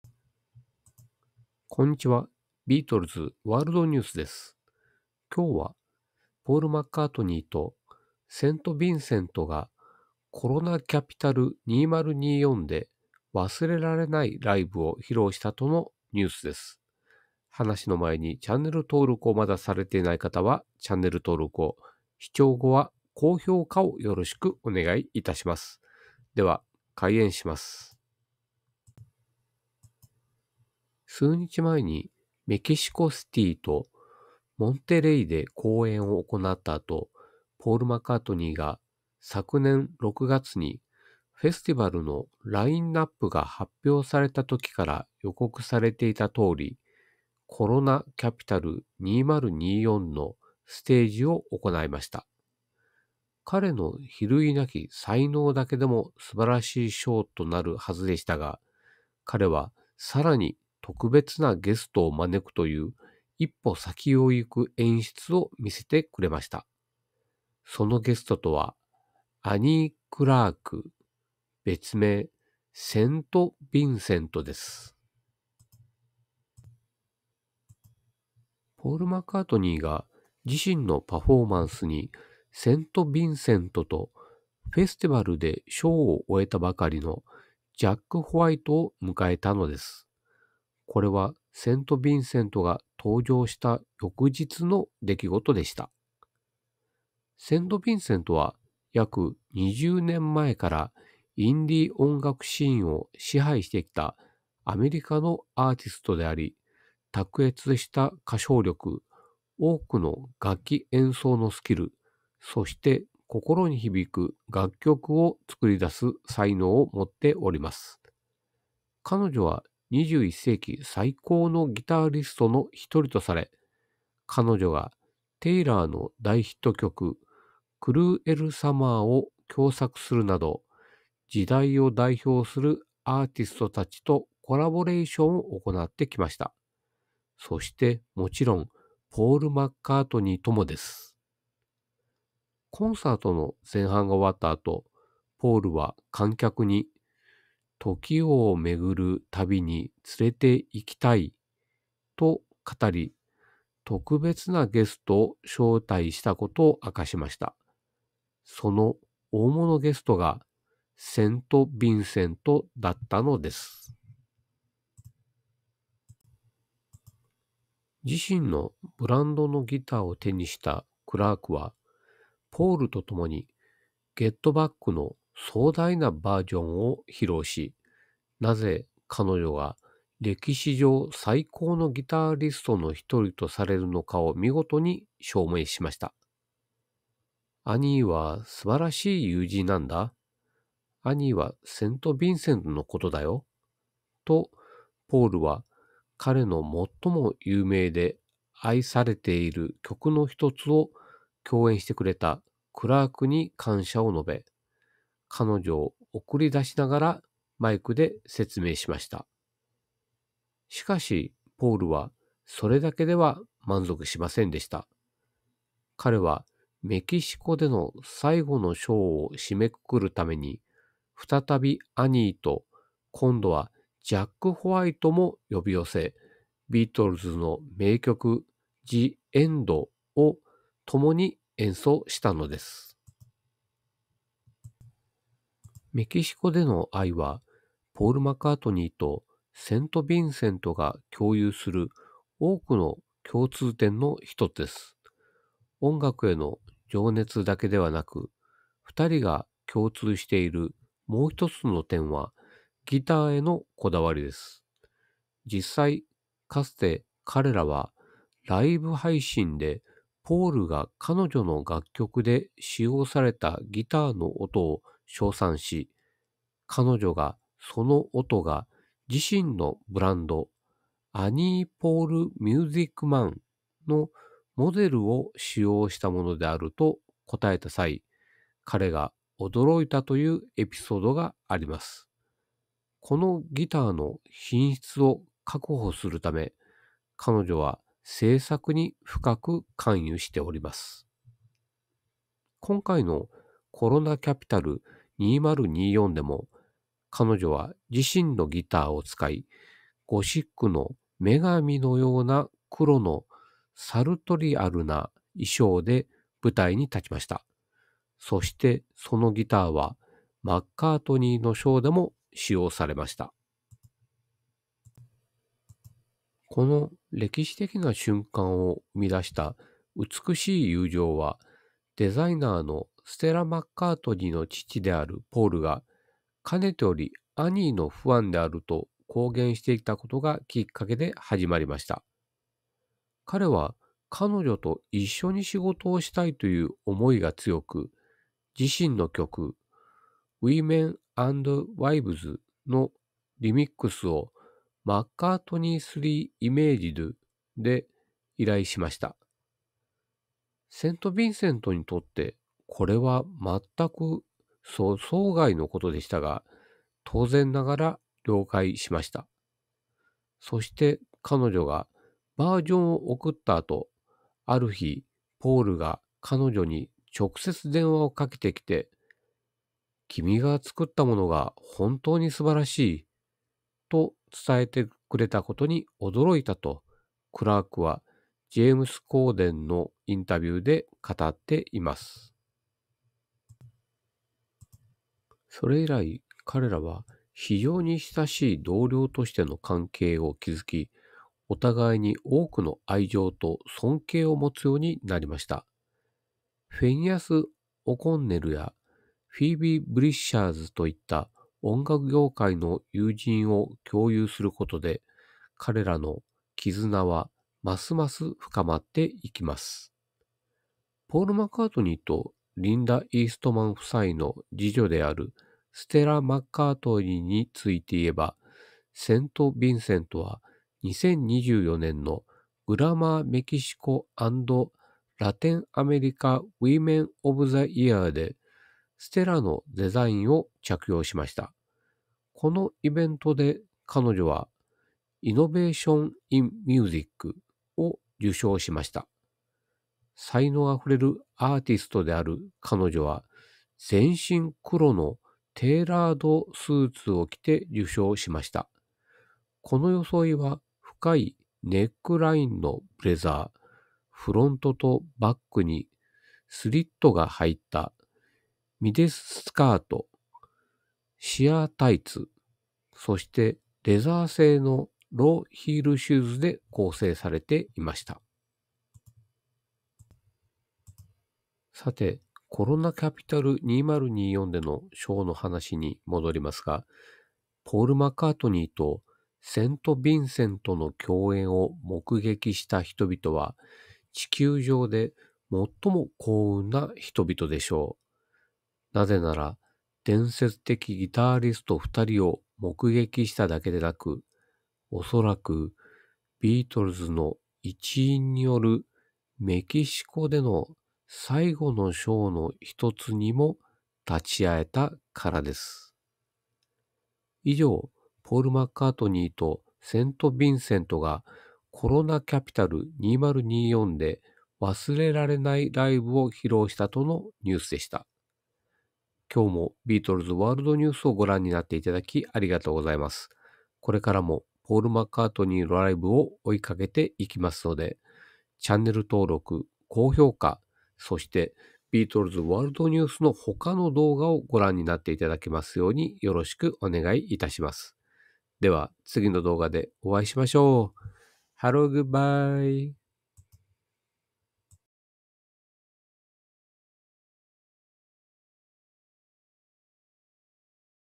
ポール・マッカートニーとセイント・ヴィンセントがメキシコ「コロナ・キャピタル2024」で忘れられないライブを披露！
数日前にメキシコシティとモンテレイで公演を行った後、ポール・マッカートニーが、昨年6月にフェスのラインナップが発表された時から予告されていた通り、「コロナ・キャピタル2024」のステージを行いました。彼の比類なき才能だけでも素晴らしいショーとなるはずでしたが、彼はさらに特別なゲストを招くという一歩先を行く演出を見せてくれました。そのゲストとは、アニー・クラーク、別名セイント・ヴィンセントです。

ポール・マッカートニーが、自身のパフォーマンスにセイント・ヴィンセントと、フェスでショーを終えたばかりのジャック・ホワイトを迎えたのです。これはセント・ヴィンセントが登場した翌日の出来事でした。

お詫び：St. Vincentをセント・ヴィンセントと発音しておりますので、ご了承願います。